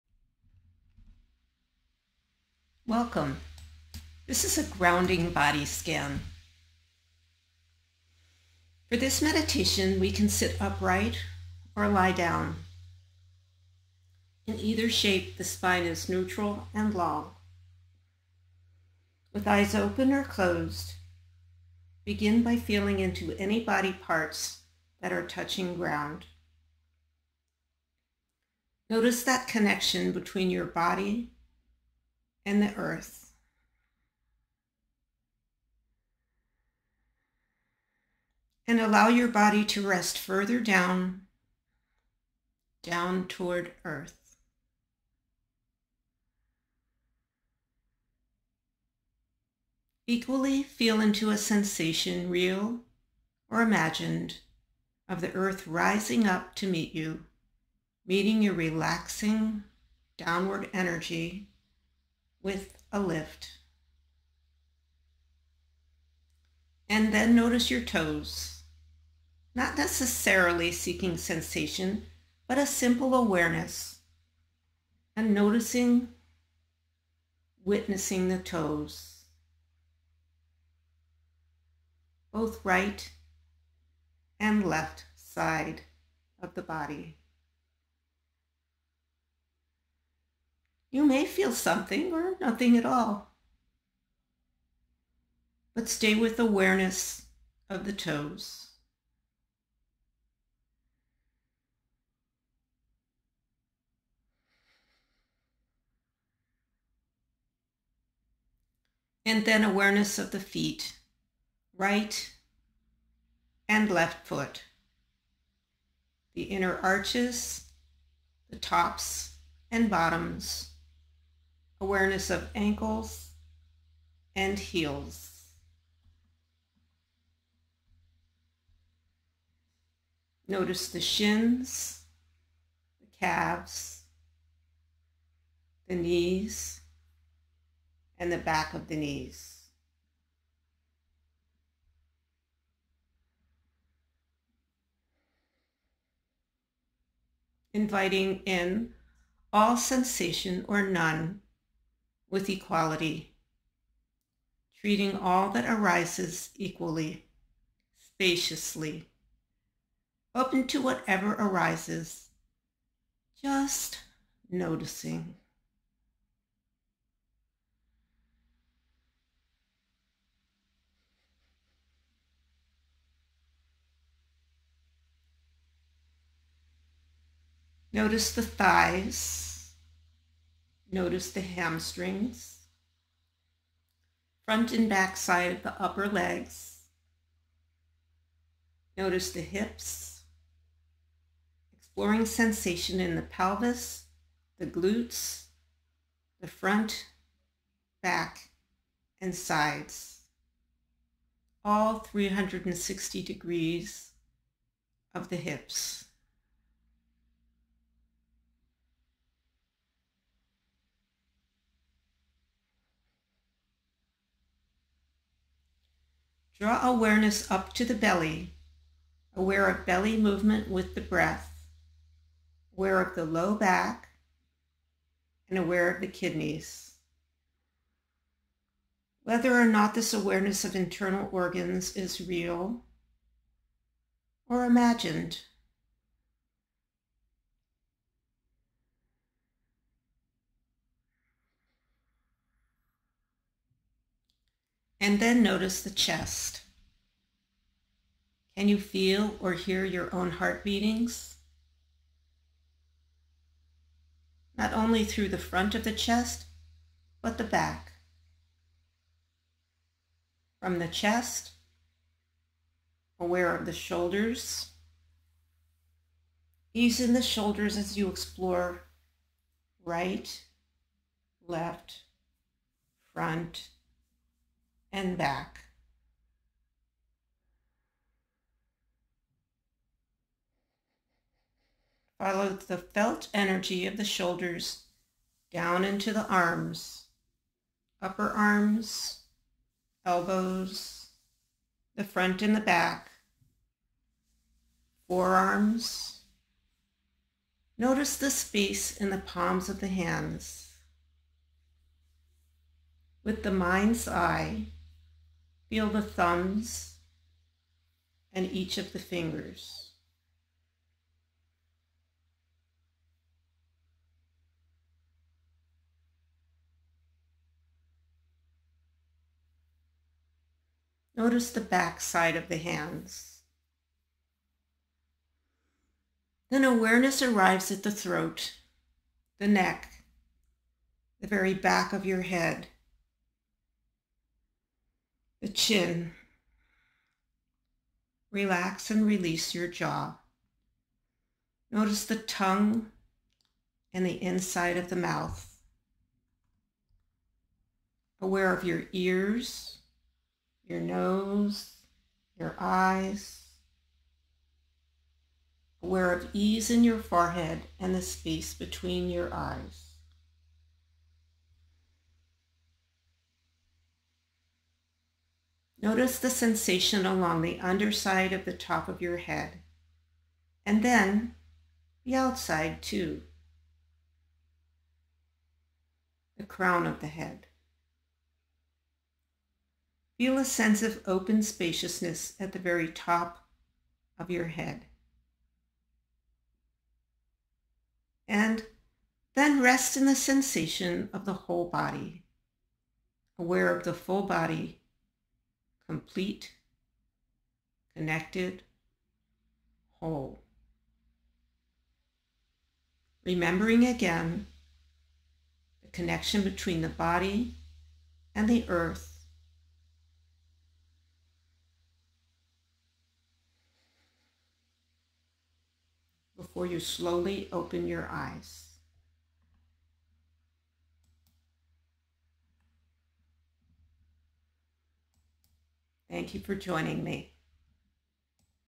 Grounding Body Scan
This practice is helpful for relaxing the body and gaining awareness of the body. It's taken from Sean Fargo's MindfulnessExercises.com